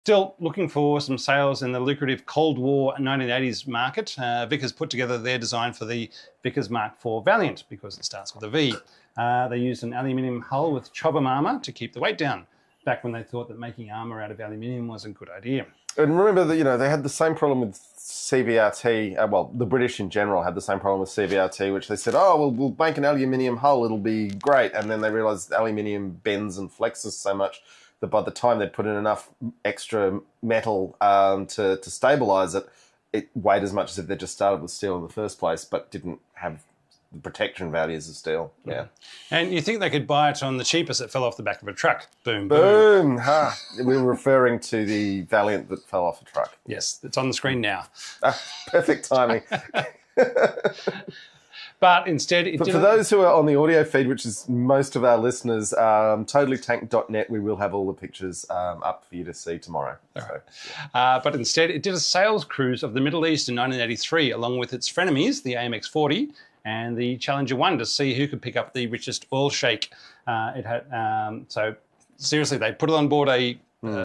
Vickers Valiant: The Cold War Tank That Fell Off A Truck
Vickers designed the Mark 4 Valiant with an aluminum hull and Chobham armor for the 1980s Cold War market. Why aluminum? They quickly learned its limitations. Tune in for the full story!